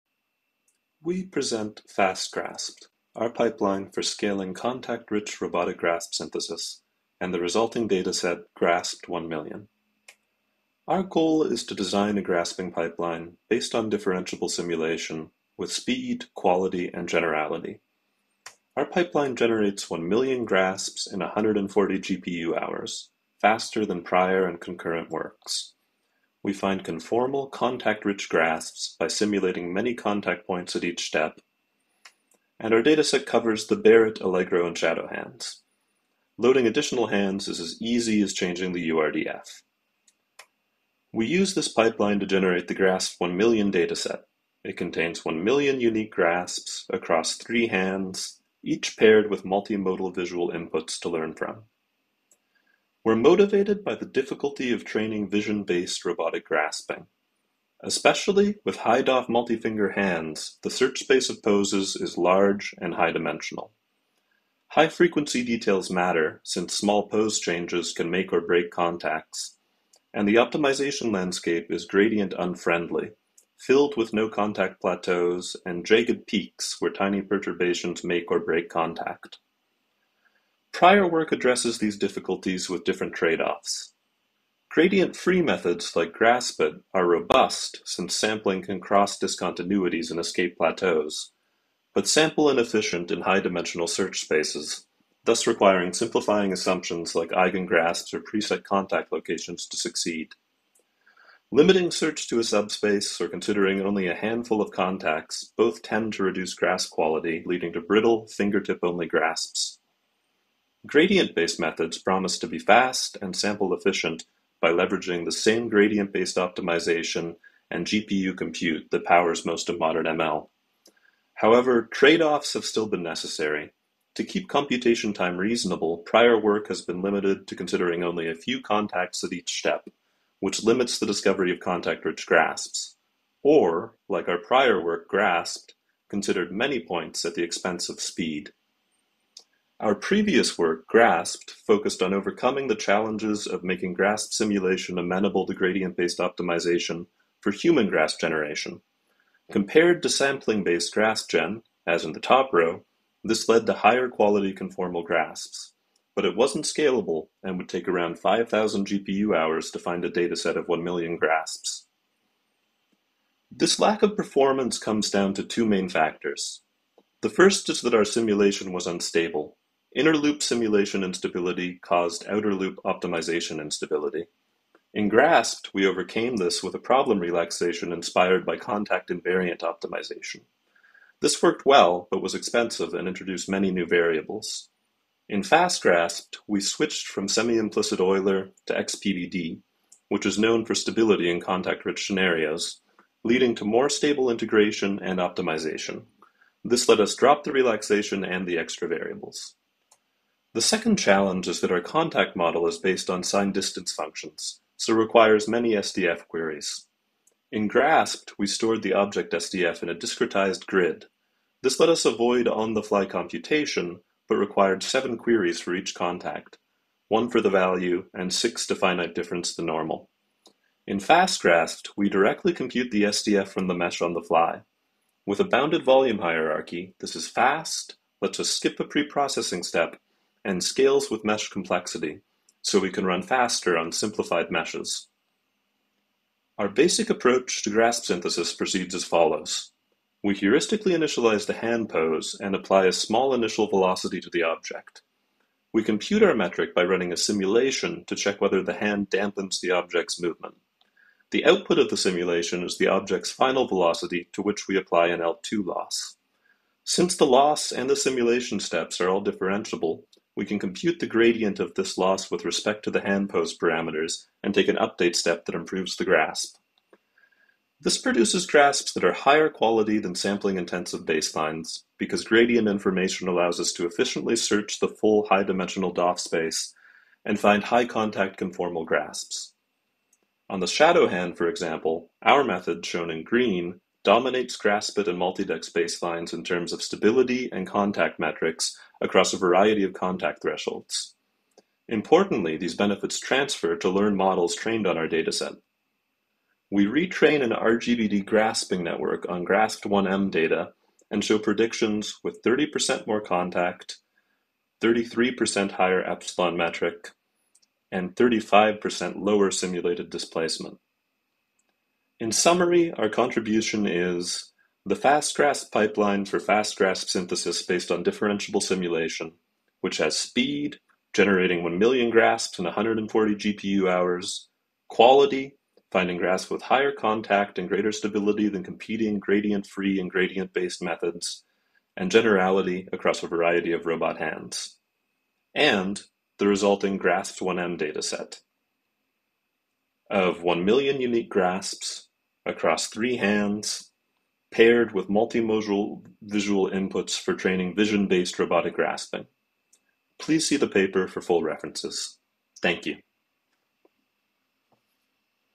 Scaling Robotic Grasp Generation through Differentiable Simulation
Fast-Grasp'D: Dexterous Multi-finger Grasp Generation Through Differentiable Simulation

Dylan Turpin, Tao Zhong, Shutong Zhang, Guanglei Zhu, Eric Heiden, Miles Macklin, Stavros Tsogkas, Sven Dickinson, Animesh Garg